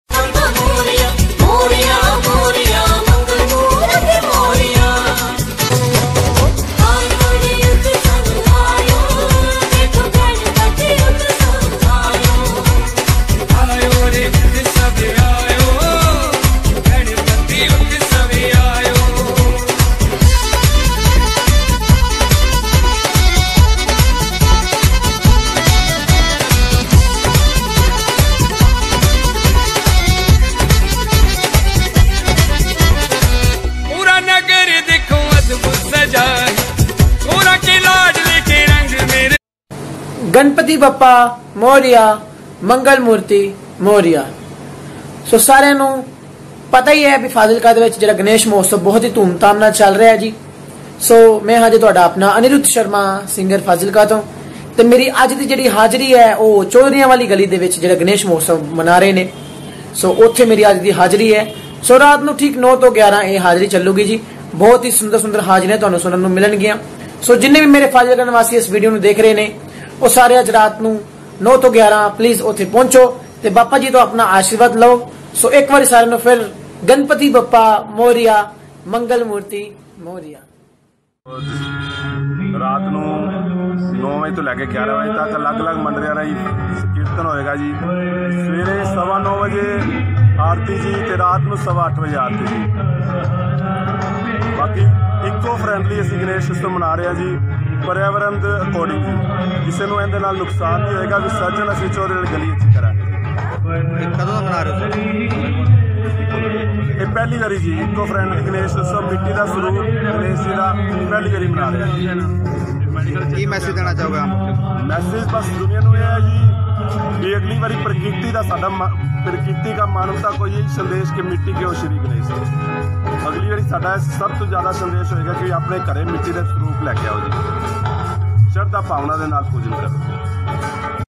FaziIka: गली चौधरियां में आज दूसरे दिन की हाजिरी लगायेंगे भजन सम्राट अनिरूद्घ शर्मा 03-09-19
RB Live Media Group of Nirog Jeevan Sansthan FaziIka